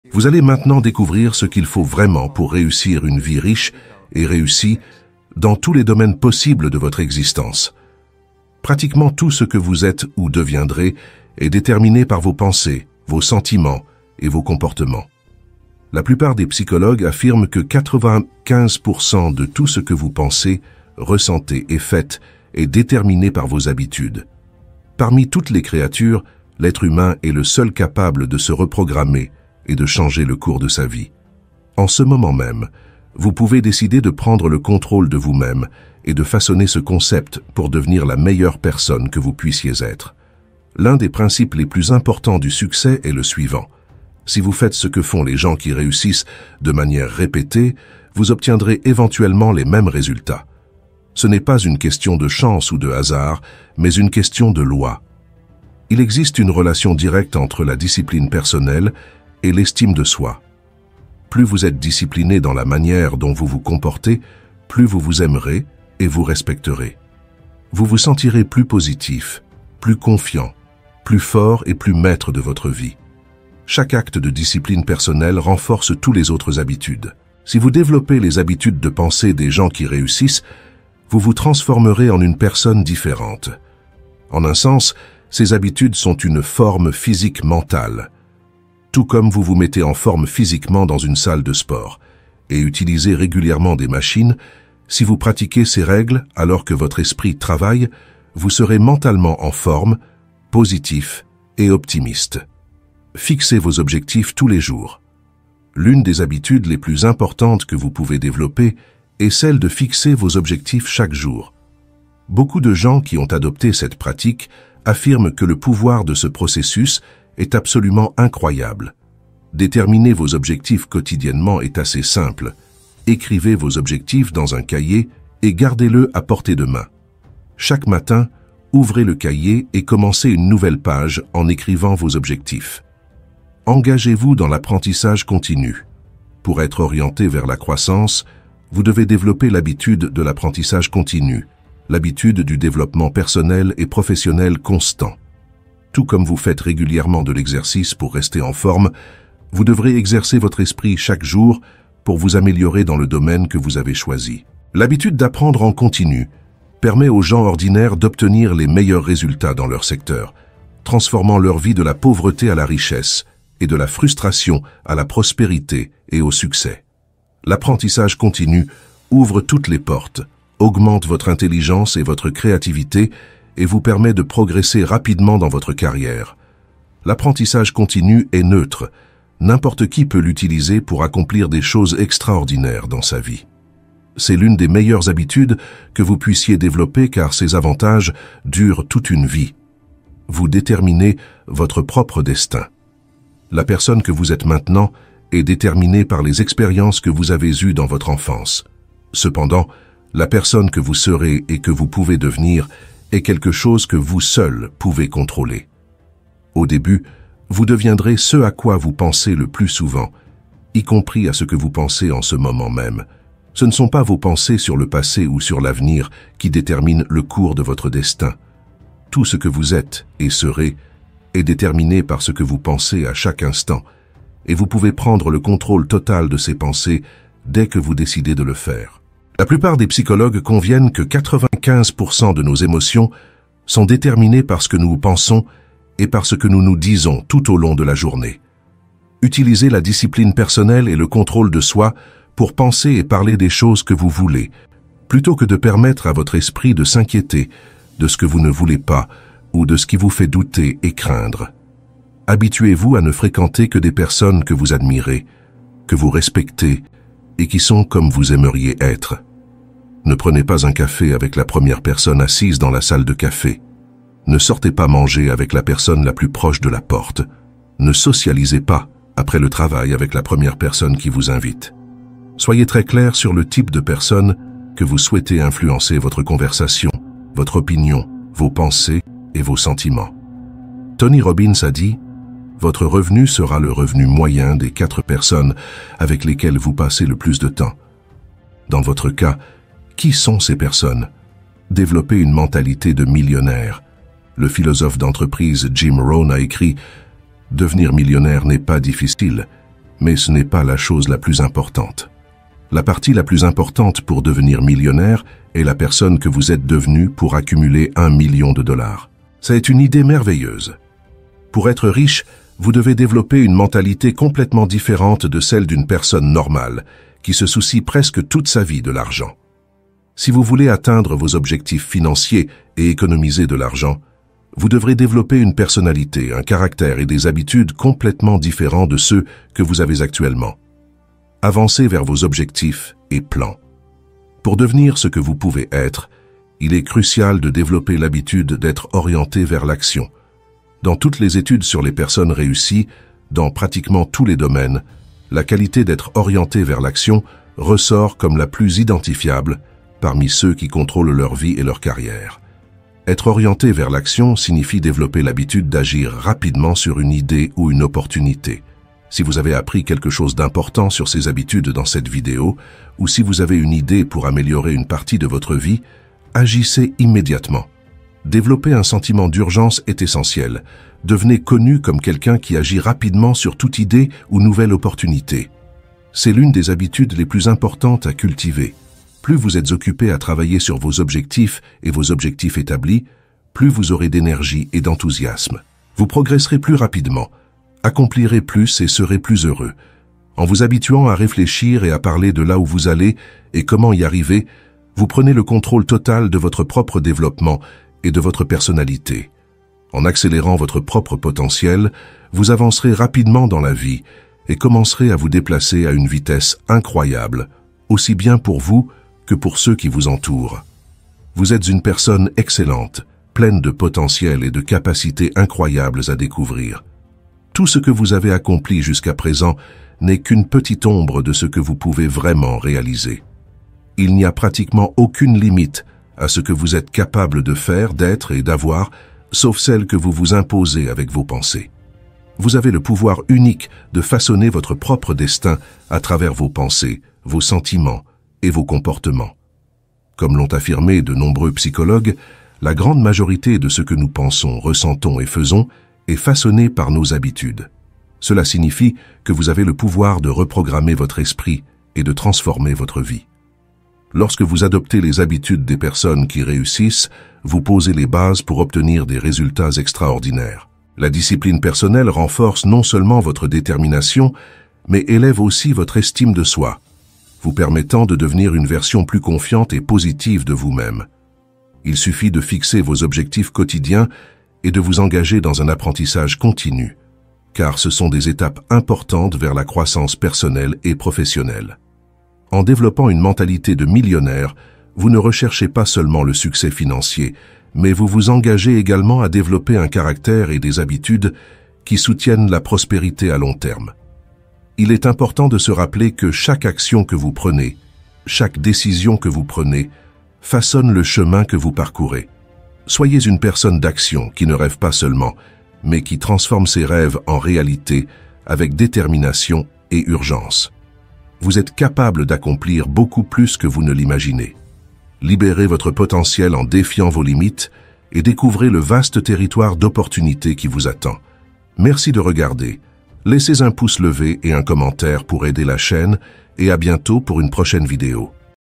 Millionnaire Montre Comment Faire de l'Argent Avec Peu d'Effort (Brian Tracy)
Découvrez comment vos pensées, sentiments et comportements façonnent votre destin, selon les psychologues. Explorez comment la discipline personnelle renforce l'estime de soi et ouvre la voie au succès. Apprenez à développer des habitudes quotidiennes de succès et le pouvoir de l'apprentissage continu pour transformer votre vie personnelle et professionnelle. Ce contenu révèle comment vous pouvez reprogrammer votre esprit, adopter une mentalité de millionnaire et agir avec détermination pour atteindre vos objectifs. Préparez-vous pour un voyage inspirant d'amélioration de soi et d'accomplissement.